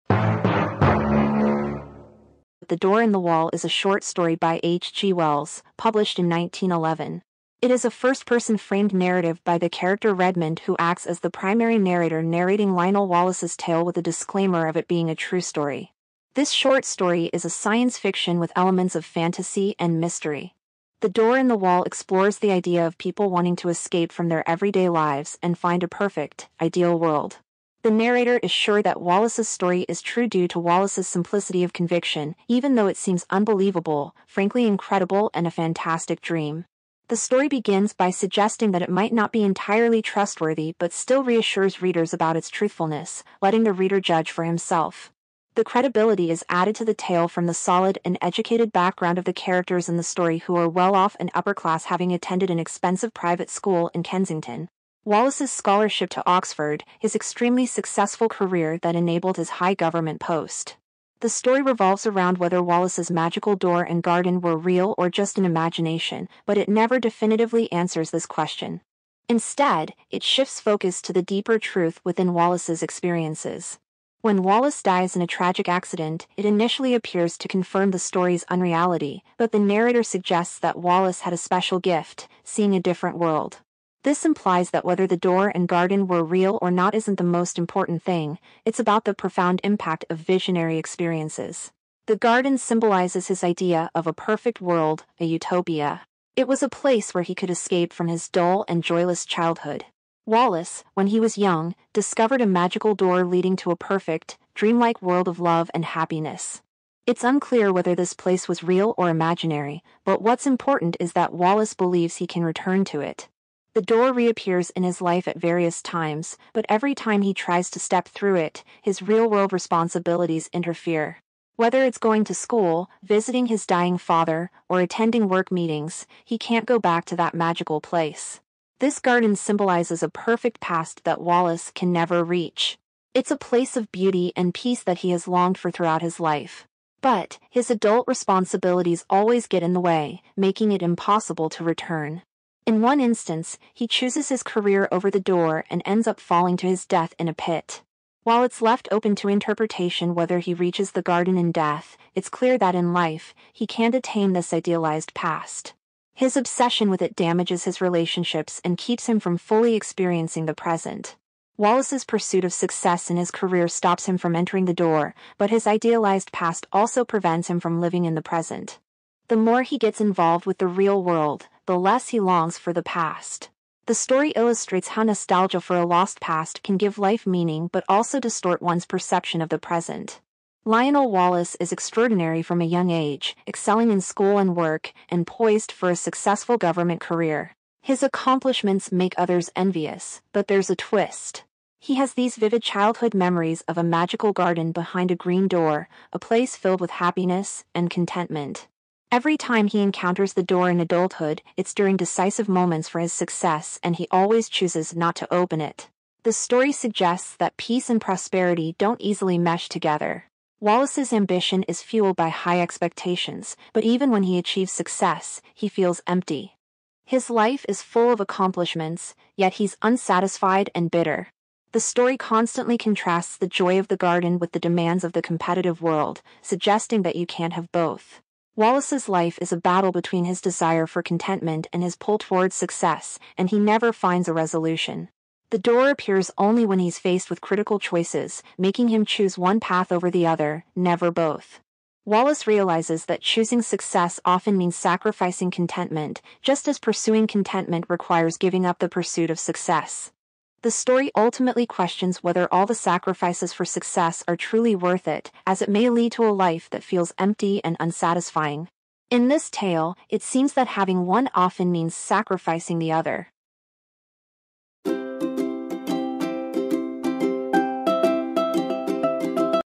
Escapism in THE DOOR IN THE WALL by H. G. WELLS Explained | Themes | Main Ideas
“The Door in the Wall” is a short story by H.G. Wells, published in 1911. It is a first-person framed narrative by the character Redmond who acts as the primary narrator narrating Lionel Wallace’s tale with a disclaimer of it being “a true story”. This short story is a science-fiction with elements of fantasy and mystery.